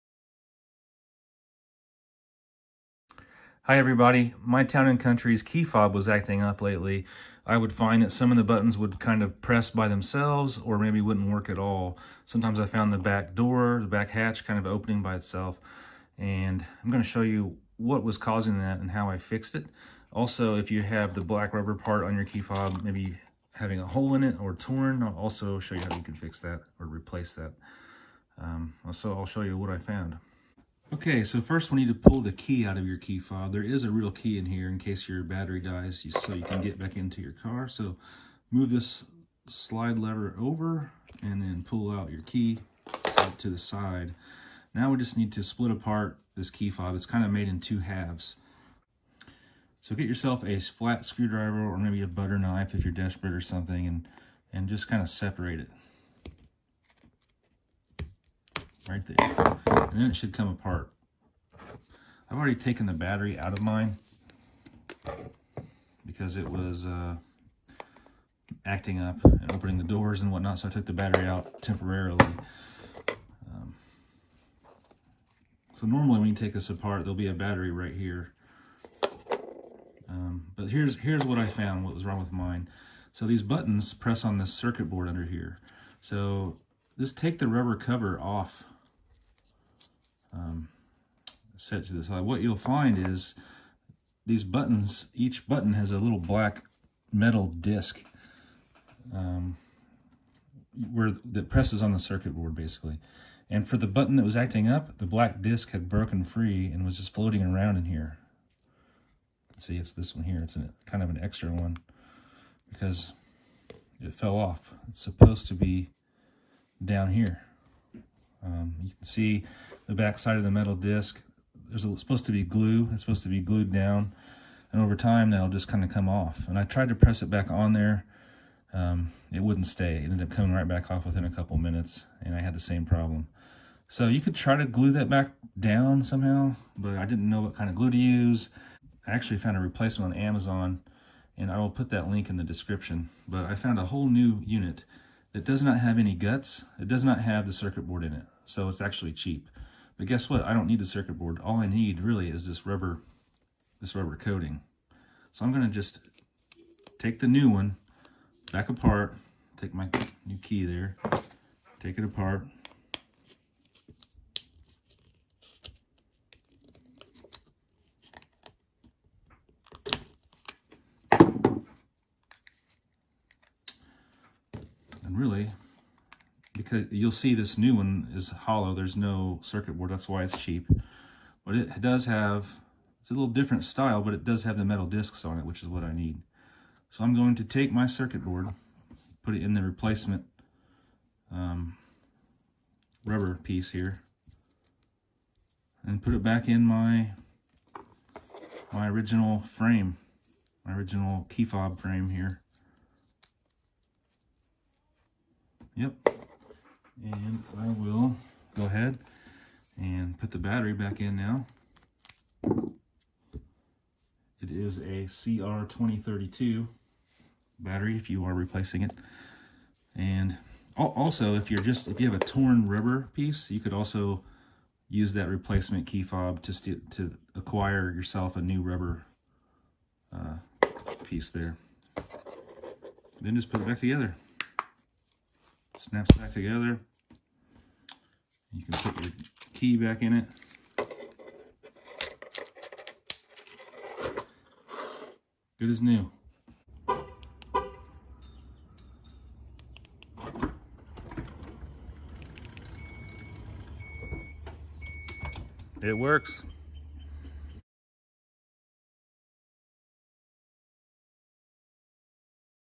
Chrysler Town & Country, Dodge, Jeep Key FOB Cheap Repair Fix. 2008 - 2016.
My van's key FOB was acting up and randomly pressing buttons by itself, so doors were being randomly locked or opened. Instead of paying big bucks to a dealer to reprogram a new remote, here is how I found the issue and fixed it.